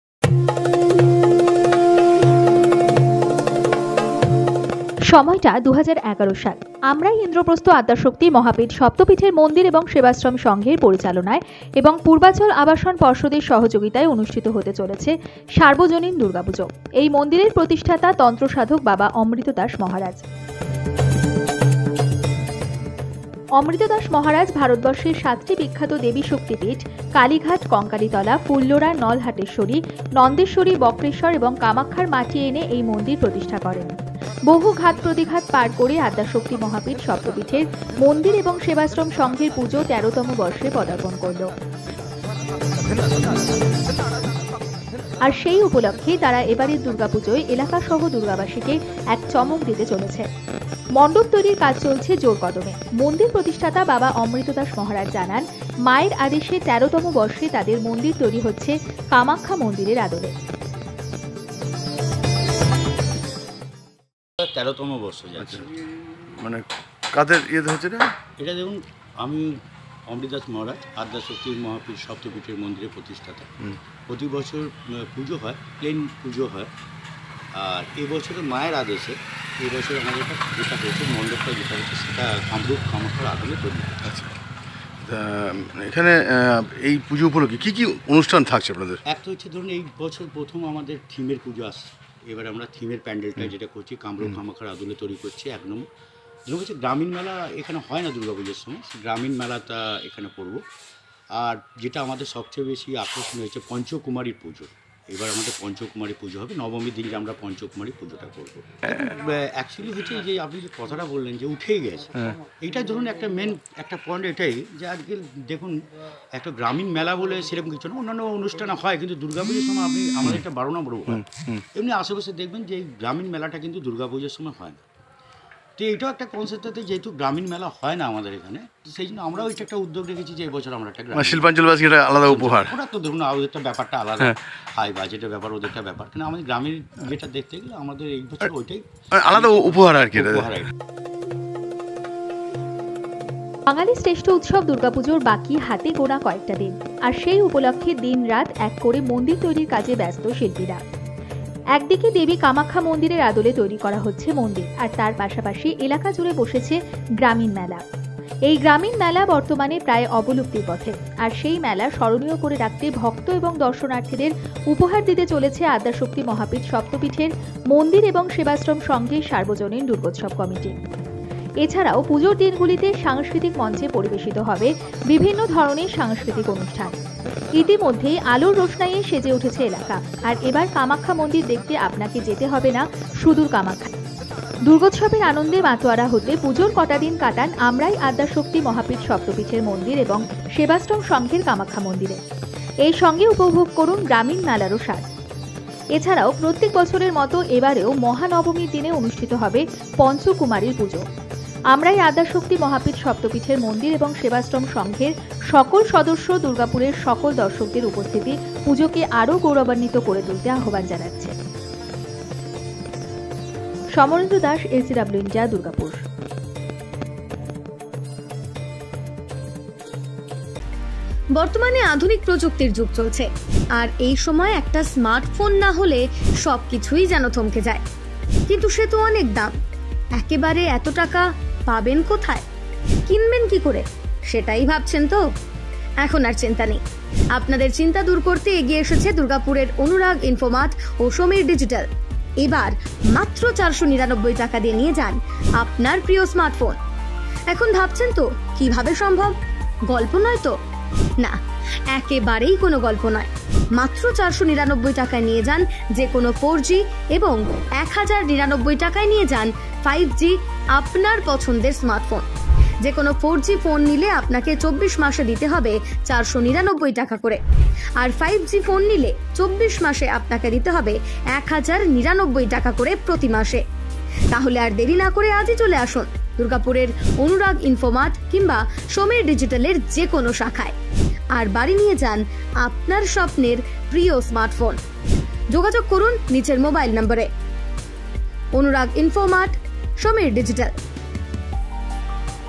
আদ্যাশক্তি মহাপীঠ সপ্ত পীঠের মন্দিরের দুর্গোৎসবের চূড়ান্ত প্রস্তুতি
কামাখ্যা মন্দিরের আদলে তৈরি হচ্ছে মন্দির, থাকছে গ্রামীণ মেলা